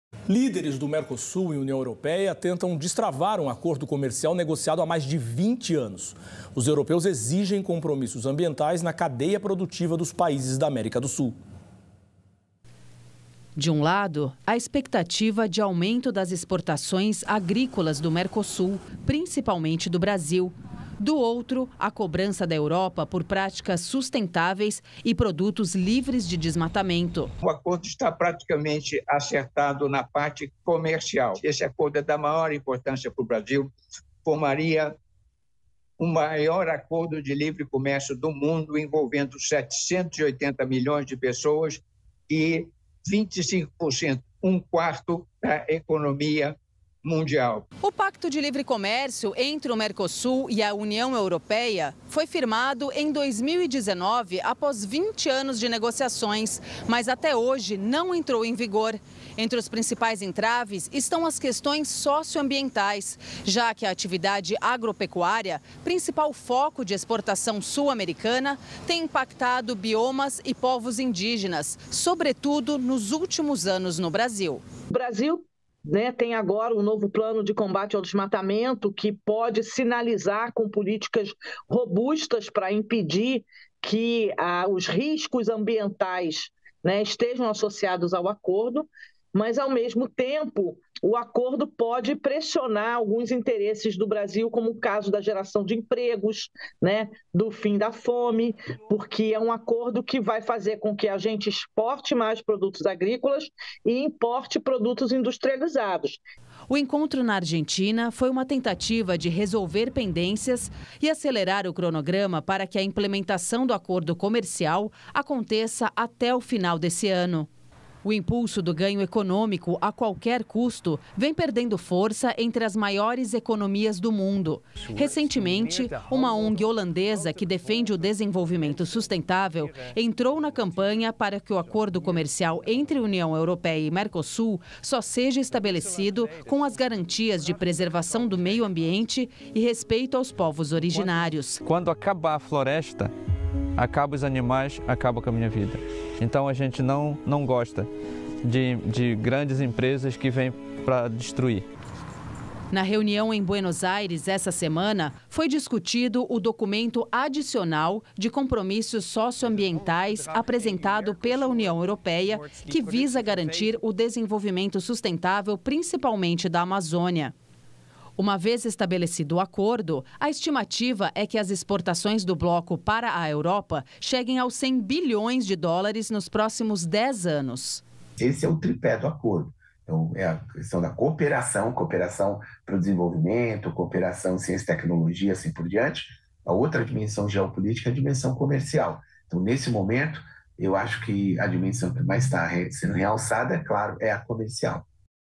Mercosul e União Europeia tentam destravar acordo comercial negociado há décadas
Líderes de Mercosul e União Europeia tentam destravar acordo comerical negociado há mais de 20 anos.

Sulamericanops têm expectativa de aumento das exportações agrícolas do bloco, principalmente do Brasil.

Europeus exigem compromissos ambientias na cadeia produtiva dos países da América do Sul.